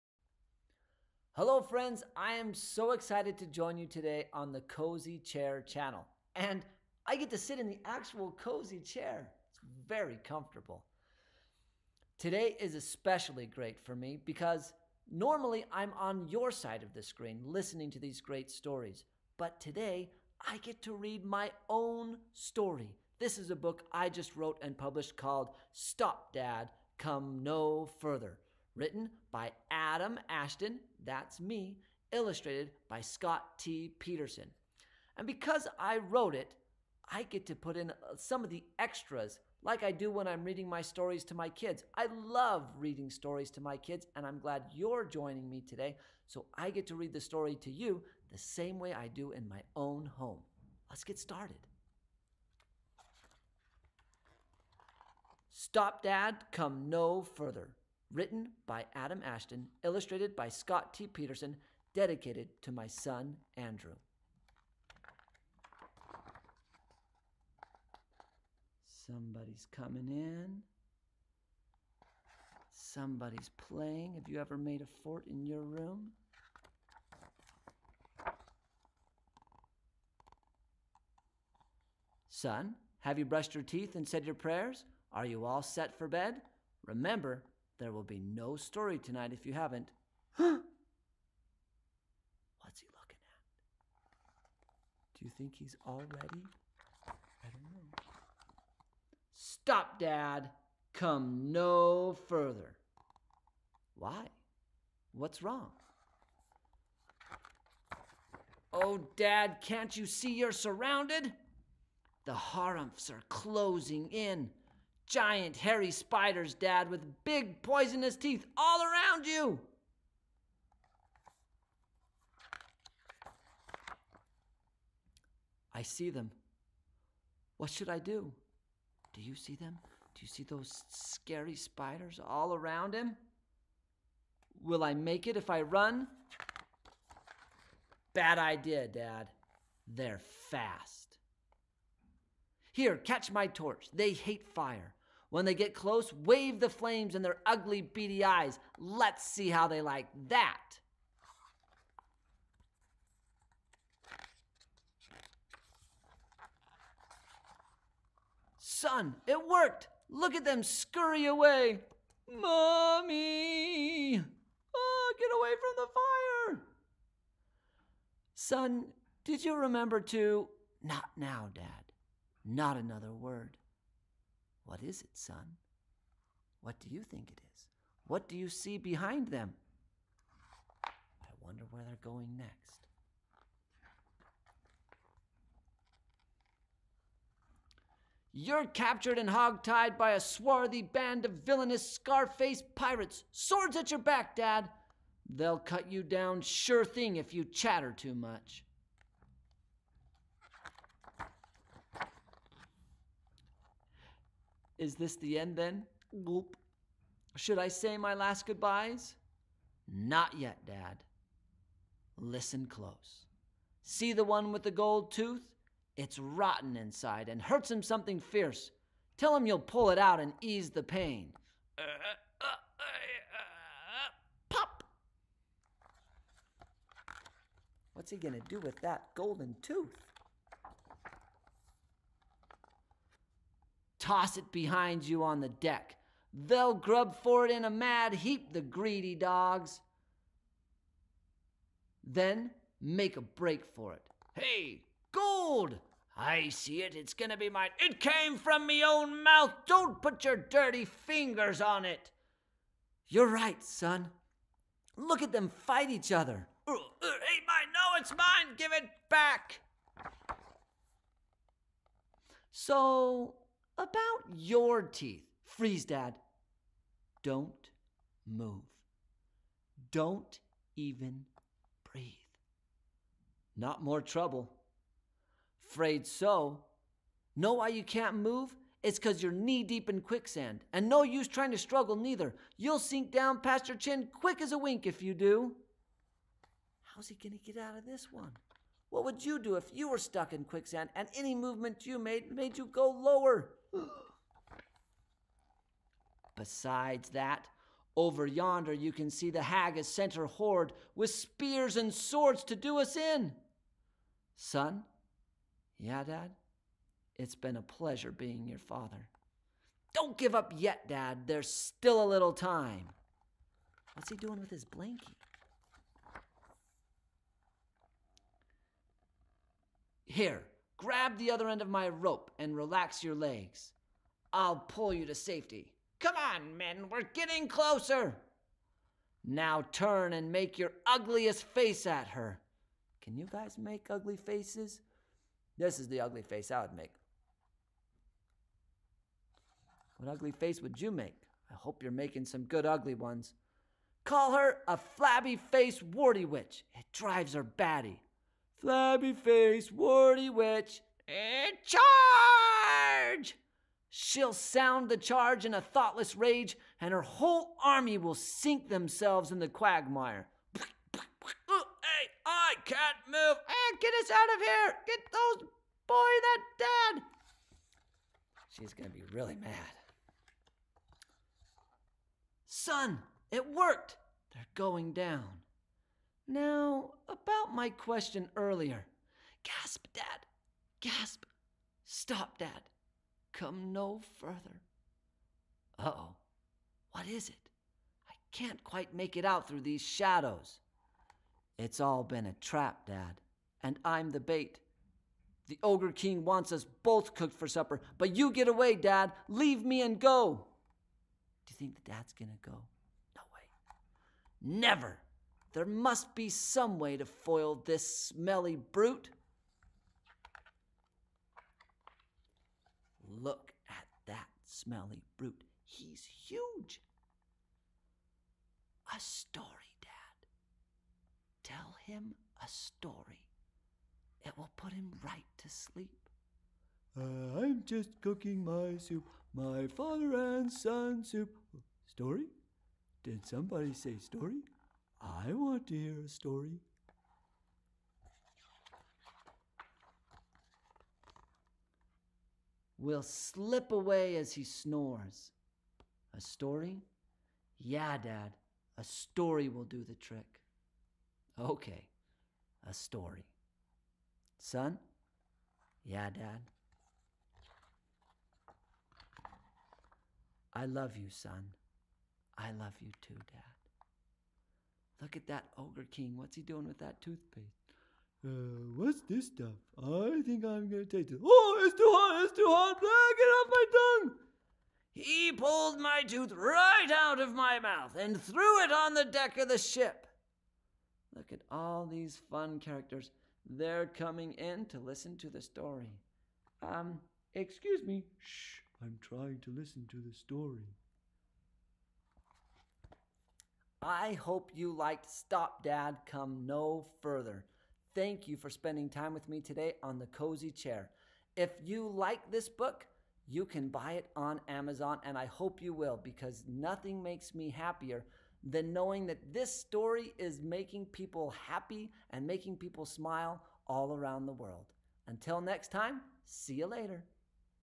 Stop Dad! Come no Further! by Adam Ashton
Welcome our guest author to The Cozy Chair! Adam Ashton shares his fascinating tale of a father and son on a dangerous journey before bed time. Come along for narrow escapes from giant spiders, pirates, and ogres. You and your child are bound to love this new bedtime favorite!

Here is the link where you can purchase the book: